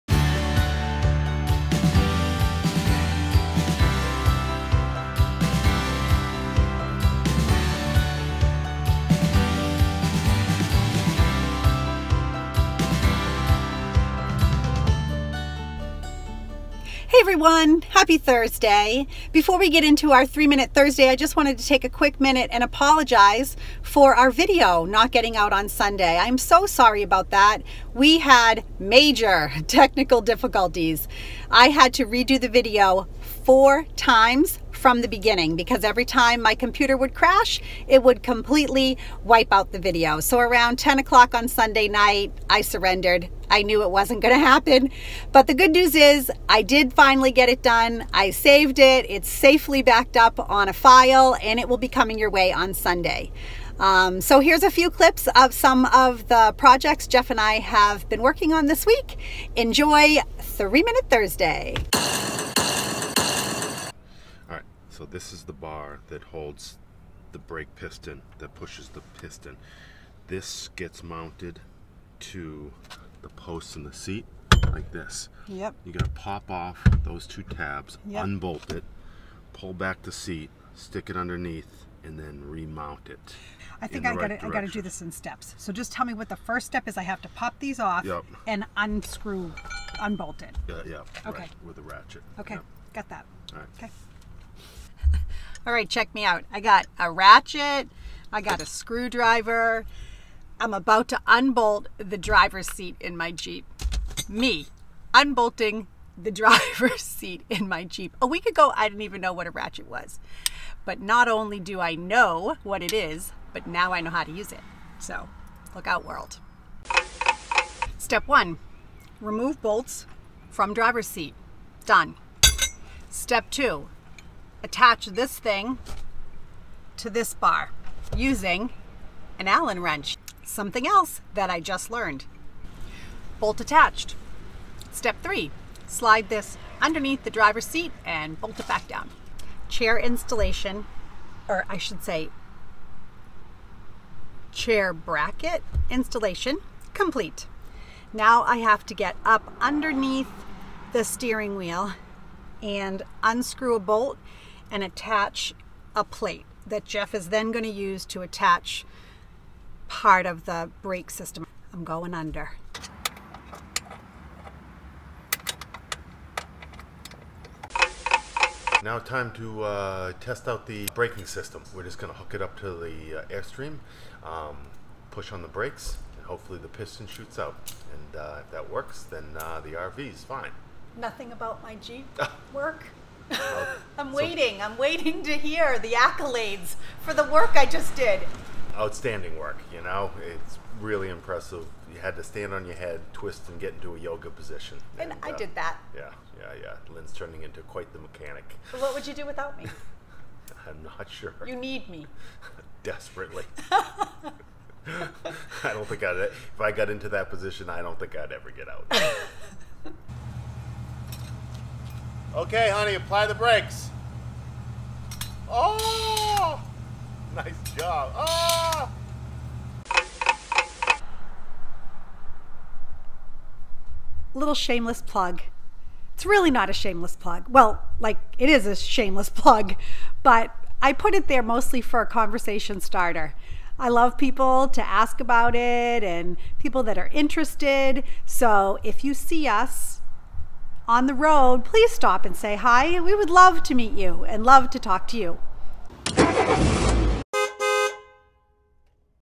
More Jeep and Airstream Project Updates - Plugging Away!
The jeep wrangler mods continue as we make progress on the Roadmaster Brakemaster and Brakeaway installation.